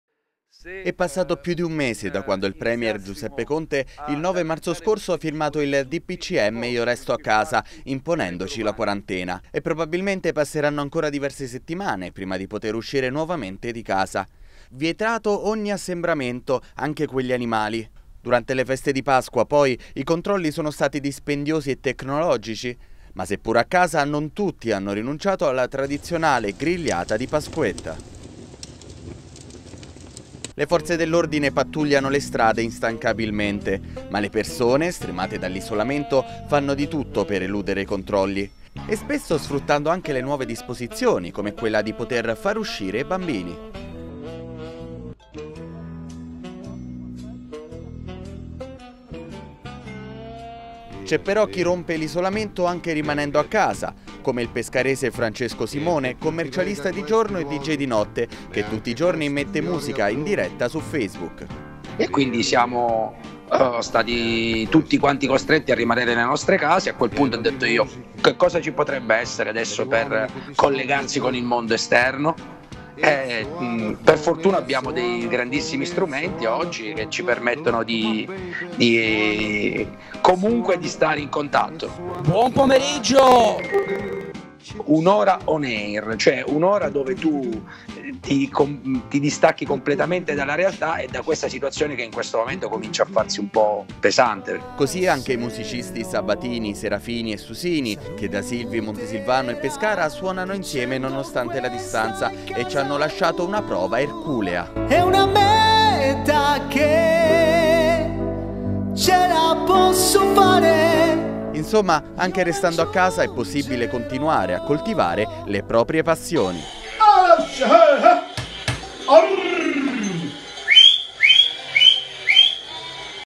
Prolungata la quarantena tra creatività e tanta ironia!
Quasi un mese e mezzo di quarantena e le persone iniziano a mostrare i primi segni di cedimento. C'è anche, però, chi si difende con la fantasia e chi rompe l'isolamento in maniera virtuale, tramite le moderne tecnologie. Ecco le perle più divertenti del web.